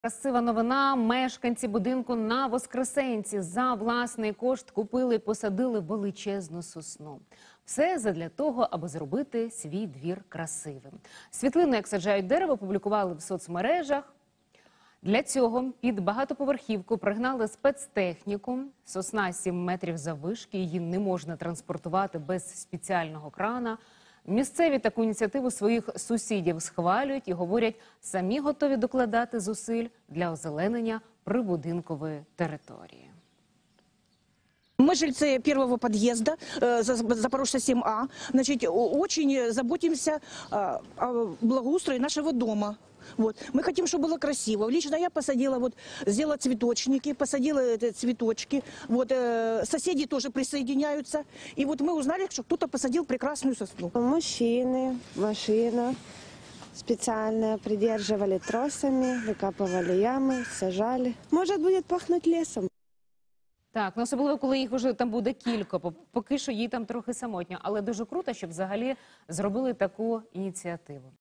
На вул. Петра Запорожця місцеві посадили величезну сосну
Місцеві посадили величезну сосну за власний кошт.